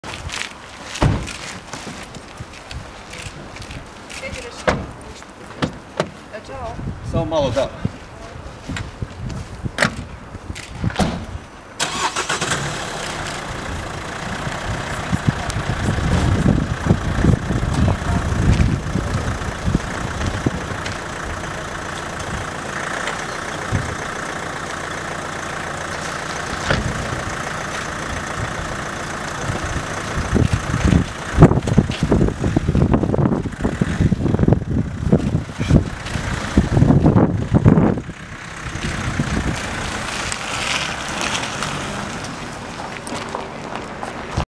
Uhicen bivsi predsjednika uprave Uljenika Gianni Rossanda
Pula, 260319.
Pocela uhicenja 12 bivsih direktora Uljanika.
 Uhicenje bivseg predsjednika uprave  Gianni Rossanda.
Snimio: Goran Sebelic / CROPIX